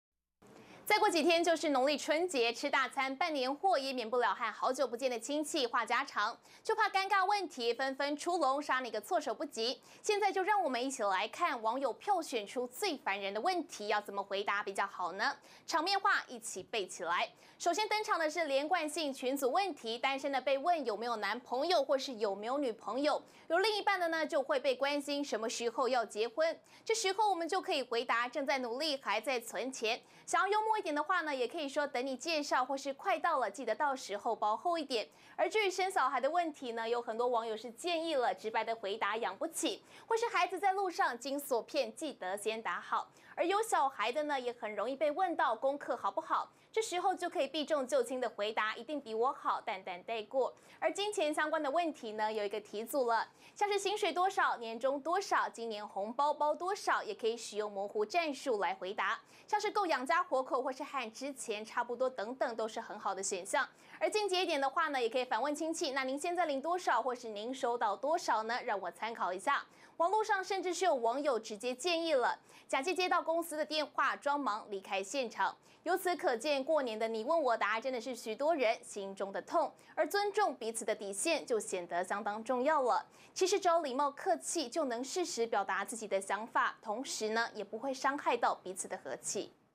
過年親戚尷聊怎麼辦? 你問我答懶人包!｜寰宇新聞20210208
逢年過節 親戚圍爐拜年免不了閒話家常，根據統計，什麼時候結婚 是很多人最害怕被”關心”的榜首問題。其他像是跟錢有關的薪水還有年終獎金等等話題，也讓不少人捏把冷汗，面對難以招架的長輩問候，該如何回應呢? 網友們有甚麼好建議?